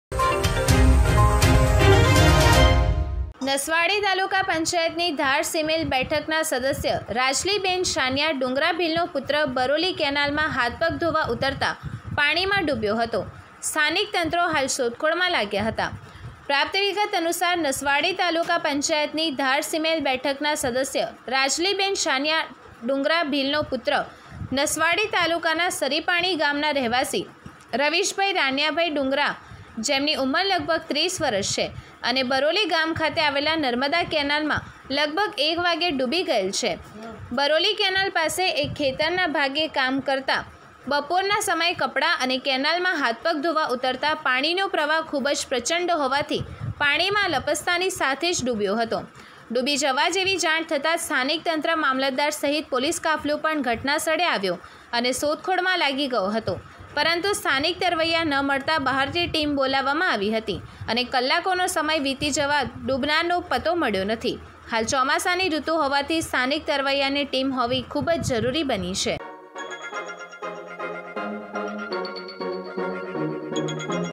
નસવાડી પંચાયત ની રાજલી બેન શાનિયા ડુંગરા ભીલનો પુત્ર બરોલી કેનાલમા હાથ પગ ધોવા ઉતરતા પાણી માં ડૂબ્યો
નસવાડી તાલુકા પંચાયત ની ધારસિમેલ બેઠકના સદસ્ય રાજલી બેન શાનિયા ડુંગરા ભીલનો પુત્ર બરોલી કેનાલમા હાથ પગ ધોવા ઉતરતા પાણી માં ડૂબ્યો .સ્થાનિક તંત્ર શોધ ખોળ માં લાગ્યું.

પ્રાપ્ત વિગતો અનુસાર.નસવાડી તાલુકા પંચાયત ની ધારસિમેલ બેઠકના સદસ્ય રાજલીબેન શાનિયા ડુંગરા ભીલનો પુત્ર  નસવાડી  તાલુકાના  સરીપાણી ગામના  રહેવાસી   રવીશભાઈ  રાનીયાભાઈ  ડુંગરા ભીલ  ઉંમર લગભગ ૩૦ બરોલી ગામ ખાતે આવેલ નર્મદા કેનાલમાં લગભગ બપોર 1:00 વાગે  ડૂબી ગયેલ છે બરોલી કેનાલ પાસે એક ખેતરના ભાગે કામ કરતો કરતો હતો બપોર ના સમયે કપડાં અને  કેનાલમા હાથ પગ ધોવા ઉતરતા  પાણીનો પ્રવાહ ખૂબ પ્રચંડ હોવાથી પાણીમાં લપસ્તાની સાથે પાણી માં ડૂબ્યો .  ડૂબી જવાની જેવી જાણ થતાં સ્થાનિક તંત્ર મામલતદાર સહિત પોલીસનો કાફલો ઘટના સ્થળે આવી ગયો અને શોધ ખોળમાં લાગી ગયો પરંતુ સ્થાનિક તરવૈયા ન મળતા બહાર થી ટિમ બોલાવામાં આવી હતી કલાકોનો સમય વીતી જતા ડૂબનારનો પતો મળ્યો નથી હાલ ચોમાસાની ઋતુ હોવાથી સ્થાનિક  તરવૈયા ની ટિમ હોવી જરૂરી છે.